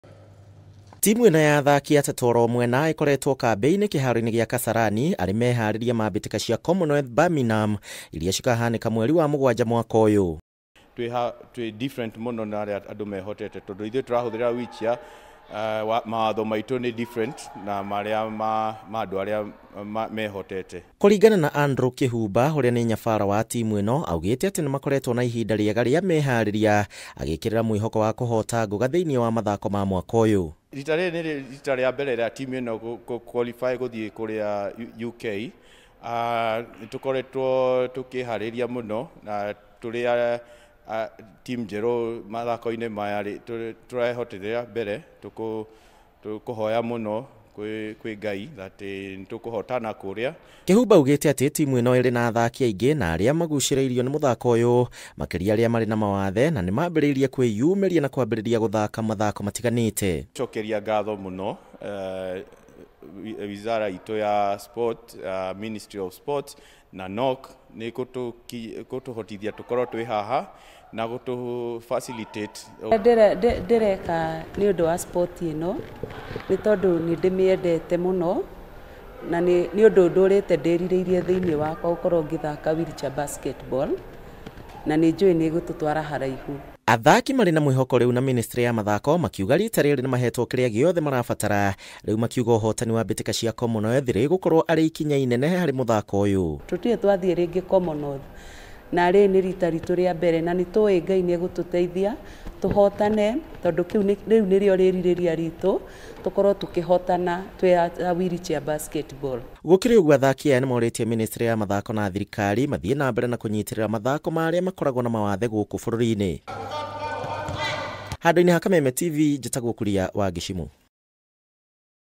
MUTHAKO WA IKABU
Timu ya kenya ya muthako wa ikabu athaki aria mari mawathe ninyitithitie thing'a mehariria ma guthie Commonwealth mweri wa mugwaja. Kocha Andrew Kihumba ari na mwihoko na athaki aake, kuria mareka mehariria kihaaro-ini gia Kasarani.